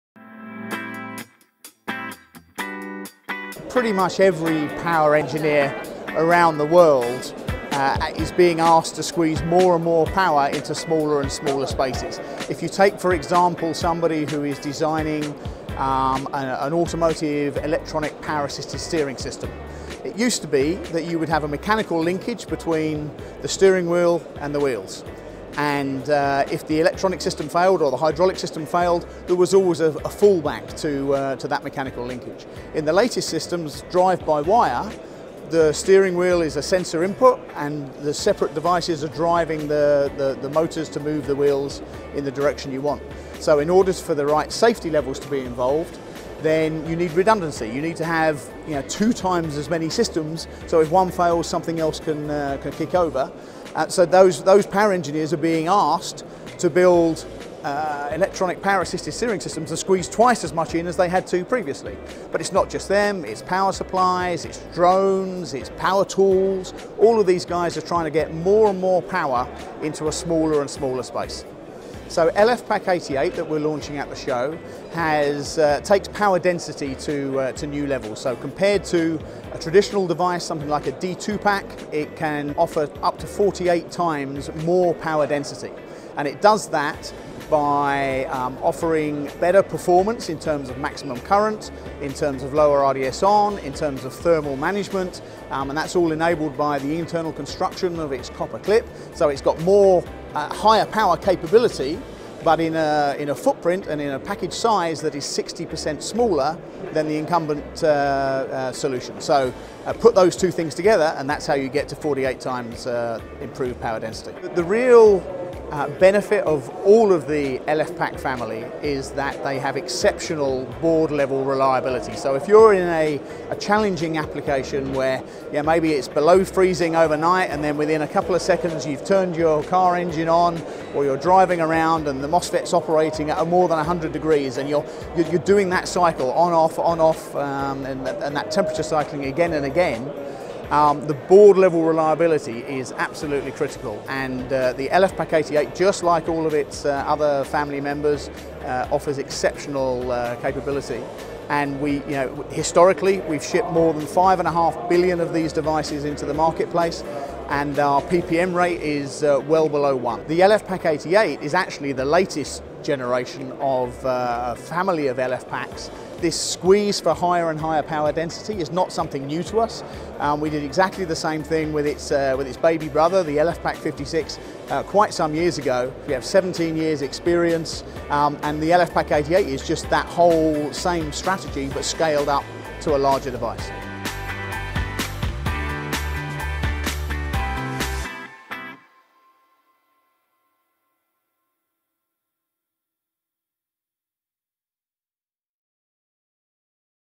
Nexperia’s new LFPAK88 MOSFET family delivers up to 48x power density
At PCIM 2019 Nexperia displays its new LFPAK88 package that, when combined with its latest silicon technology, results in 40 V MOSFETs delivering a low RDS(on) of 0.7 mΩ. Being interviewed by Electronics Weekly Chris Boyce, Marketing Director at Nexperia, explains how LFPAK88 devices are an ideal replacement for larger power packages such as D²PAK and D²PAK-7, providing up to 48 times power density. And, by measuring 8 x 8mm they offer a footprint reduction of 60%, and a 64% lower profile.